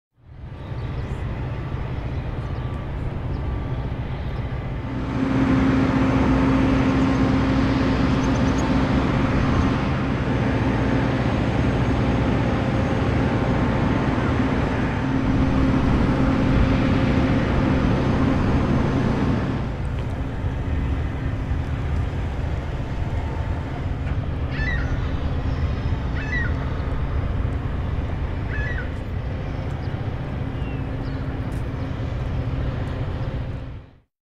USS Minneapolis-St. Paul will be in Cleveland until June 1
The USS Minneapolis-St. Paul, littoral combat ship 21, is docked in front of FirstEnergy Stadium on the waterfront of the Cleveland-Cuyahoga County Port.